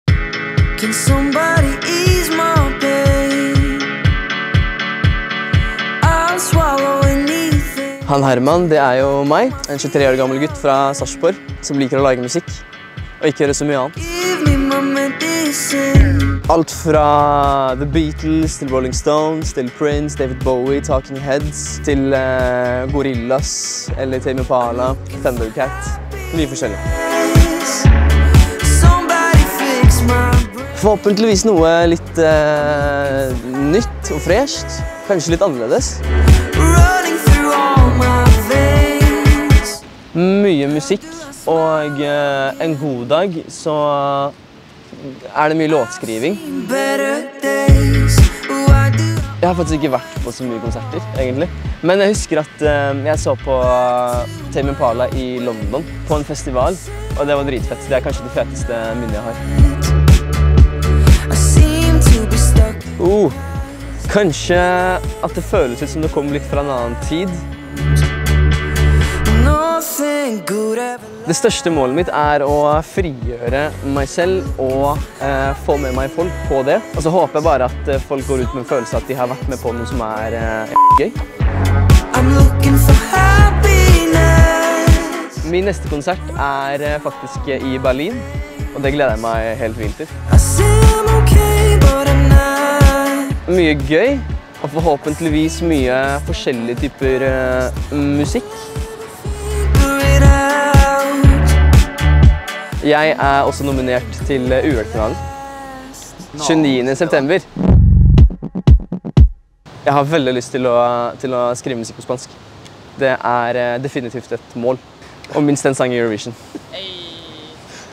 Bli kjent med HAN HERMAN!
Bli kjent med våre artister i serien "Bli kjent med". Denne gangen er det Han Herman.

Regi/Foto/Klipp: Stian Werme